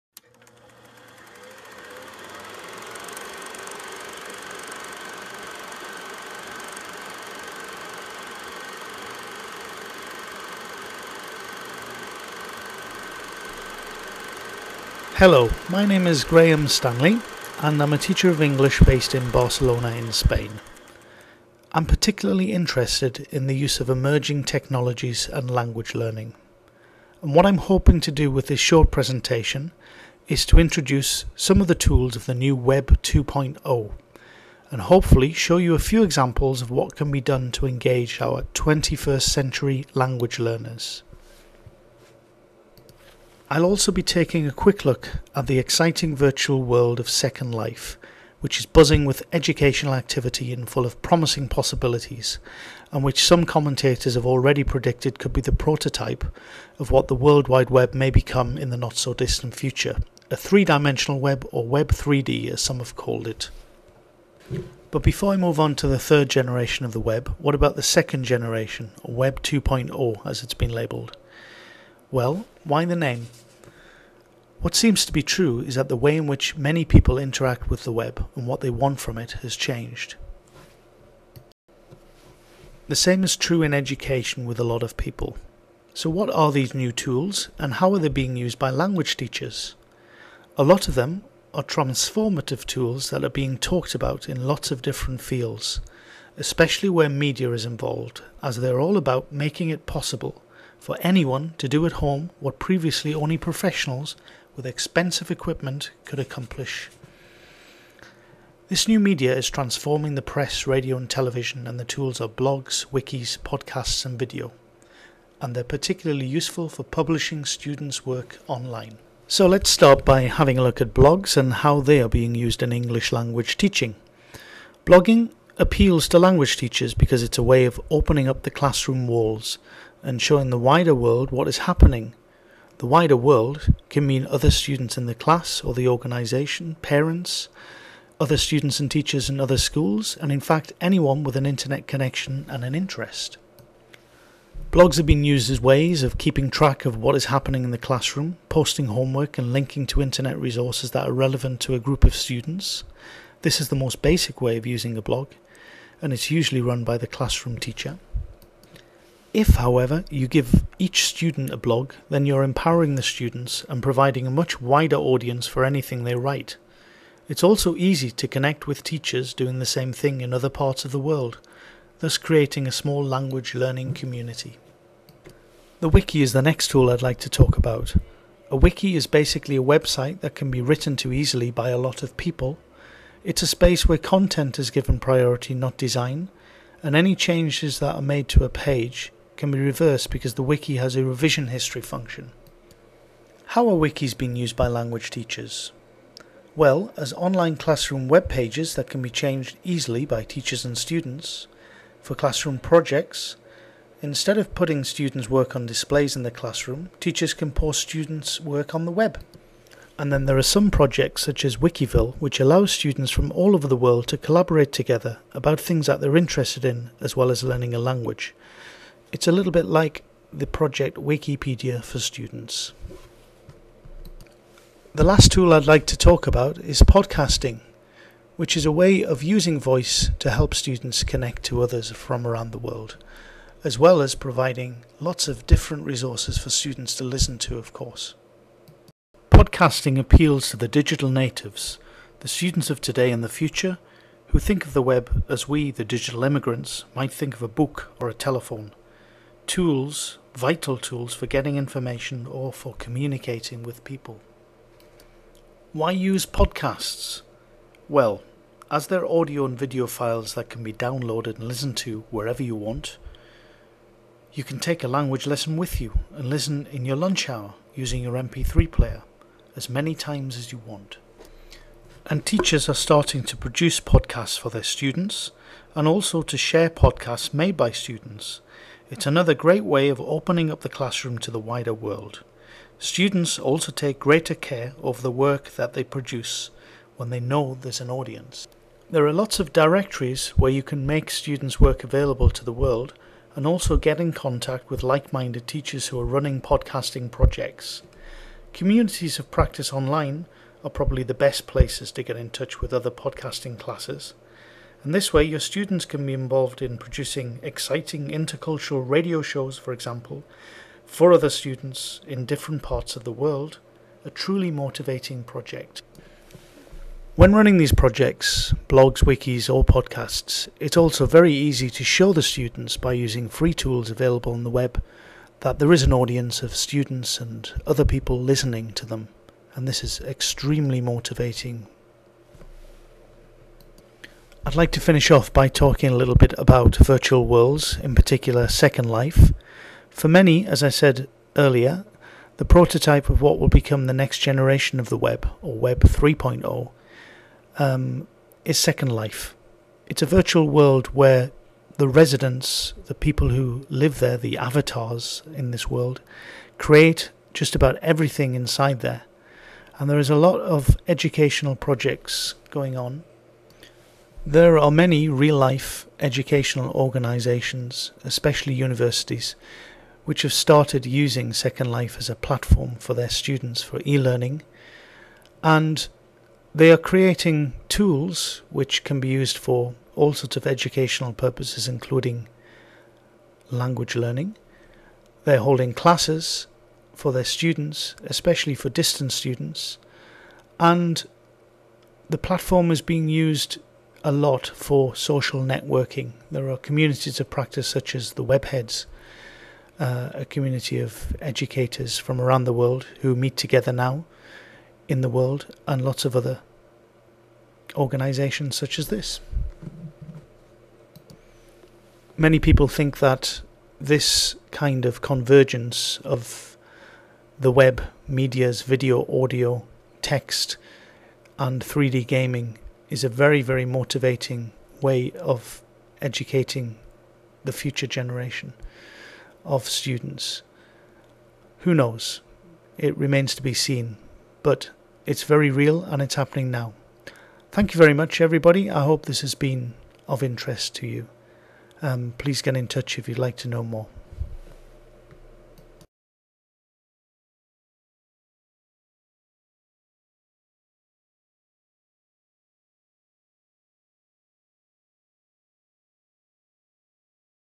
Web 2.0 & Language Learning
Presentation of Web 2.0 for Educators interested in language learning and emerging technologies